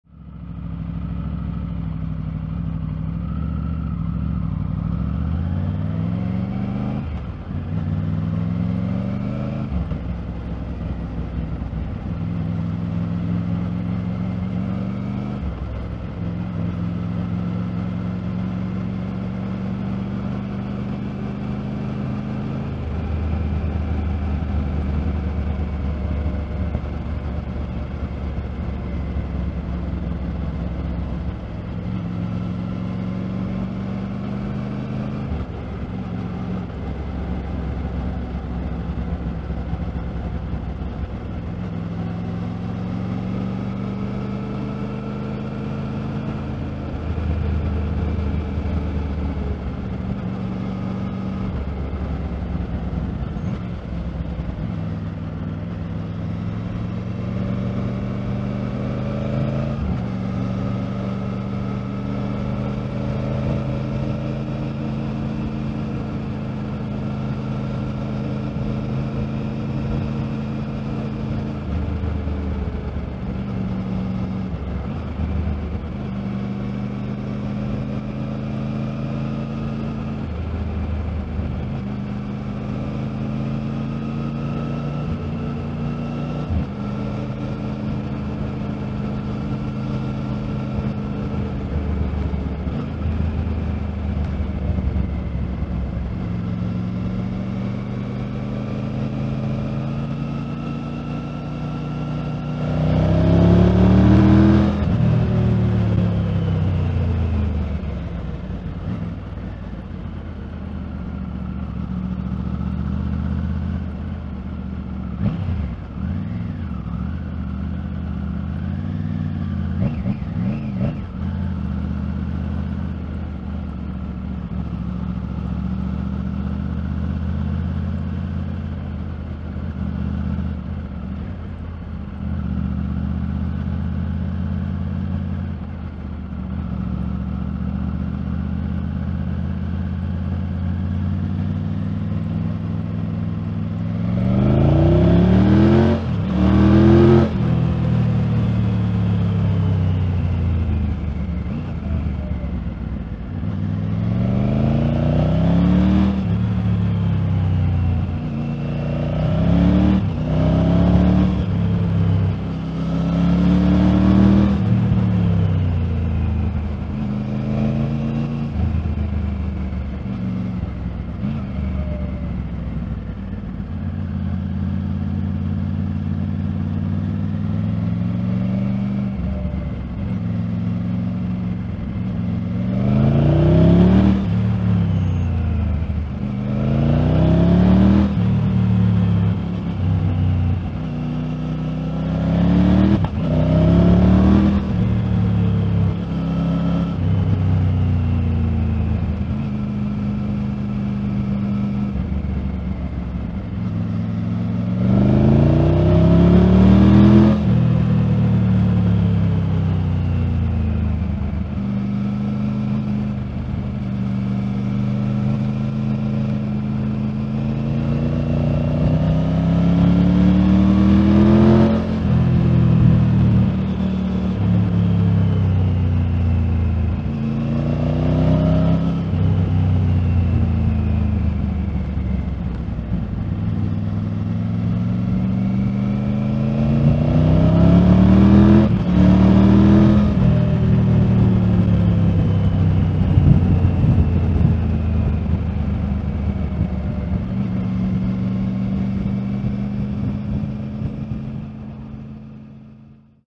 Best coastal road in South Africa: Chapman's Peak Drive, Cape Town
Chapman's Peak Drive is a favourite coastal drive of many South African bikers and tourists alike. This ride is from the Noordhoek side, riding towards Hout Bay. Video was captured with Insta360 X3 and audio with DJI Wireless Mic2.